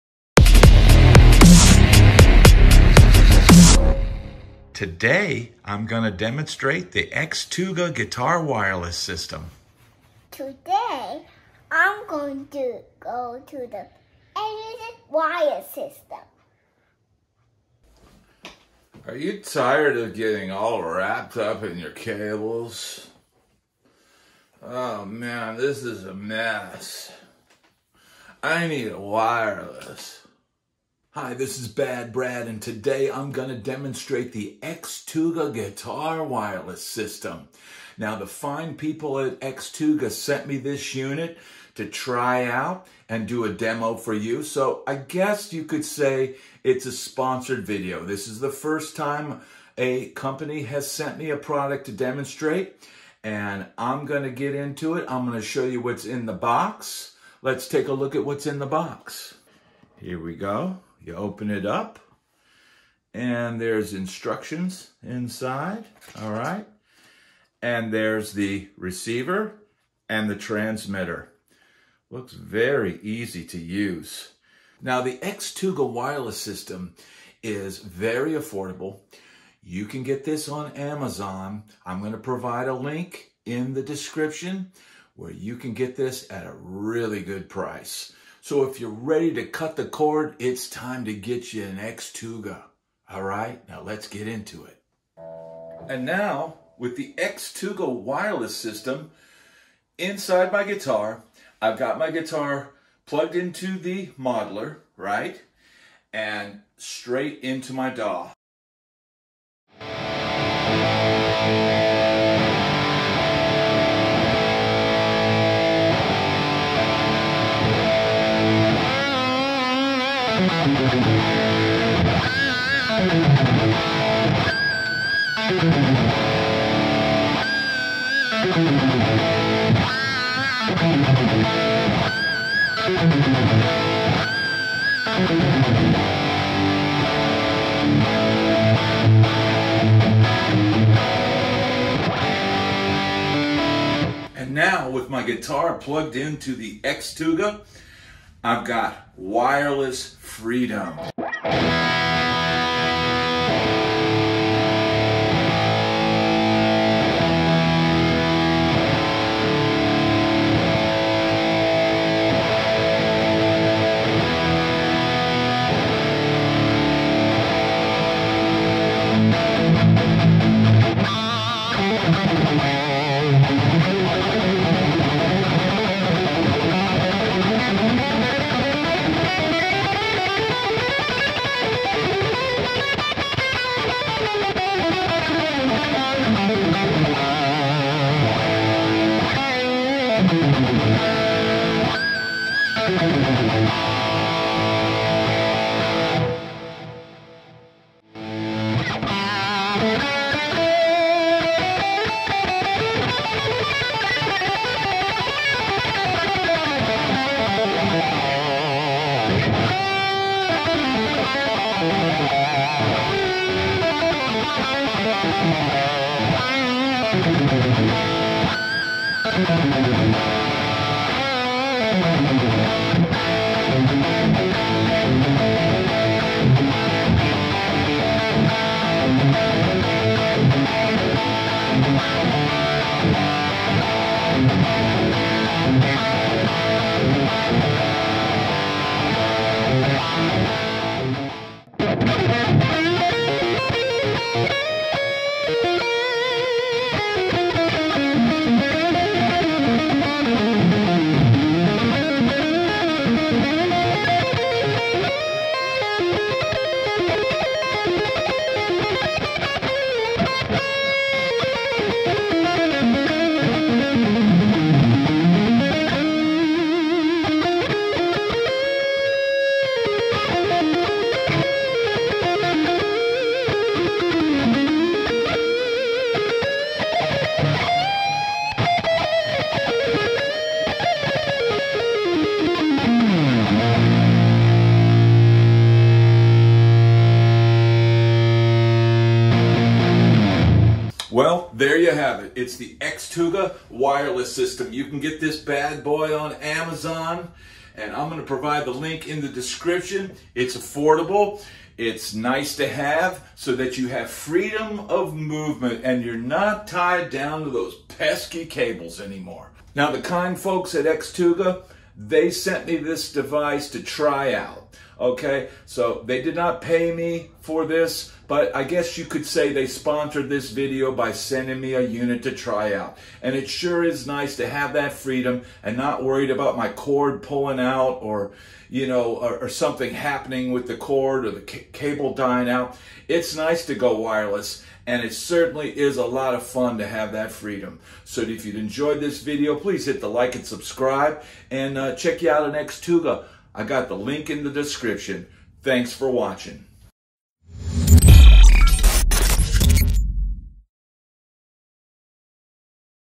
Unleash Your Guitar's Potential: XTUGA JTSY23 Wireless Guitar System Demo!
You can pick up the Xtuga Wireless System

Welcome to the ultimate liberation for guitarists - the XTUGA JTSY23 Wireless Guitar System! 🎸 Say goodbye to the constraints of cables and experience true freedom on stage or in the studio. In this demo, we unbox the XTUGA JTSY23 to reveal its sleek components and show you exactly what you get in the package.

But that's just the beginning! We'll plug in this game-changing wireless system and pair it effortlessly with a pristine PRS Custom 24 and a Line 6 Pod Express. Prepare to witness the seamless connectivity and impressive sound quality that the JTSY23 offers.

Watch as we embark on a cable-free journey, exploring the versatility and mobility this system brings to your performance. From rocking' riffs to blistering solos, we'll demonstrate how the XTUGA JTSY23 empowers you to unleash your creativity without limitations.

Join us as we rock out in style, breaking free from the tethers of traditional guitar setups and embracing the future of wireless innovation. Get ready to elevate your playing experience with the XTUGA JTSY23 Wireless Guitar System! 🚀🎶